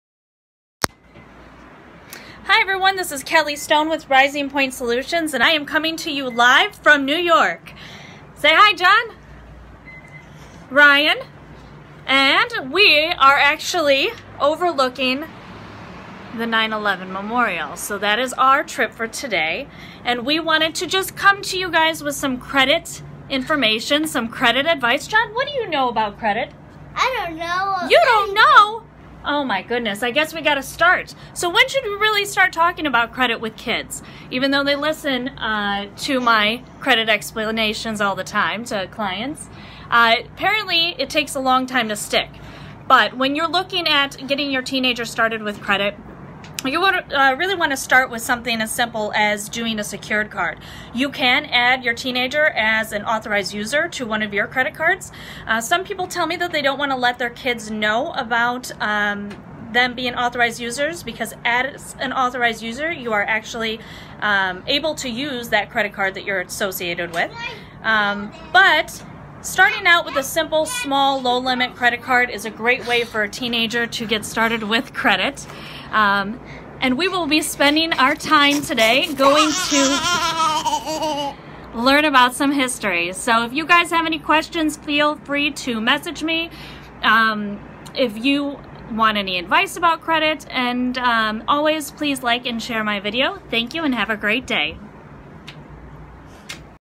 How to teach your teenager about credit
Not many people know how or where to start teaching teenagers about credit building. However, the first step is to establish a secured card.